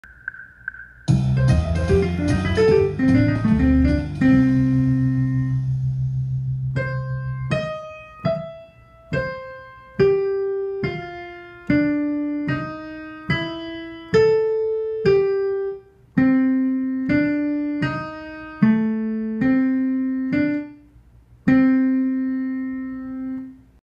Jazz piano lick #38 - Bluesy Turnaround
Jazzpianoschool.com/lotd38 for notated music

This bluesy turnaround lick can be used pretty much anywhere. Enjoy!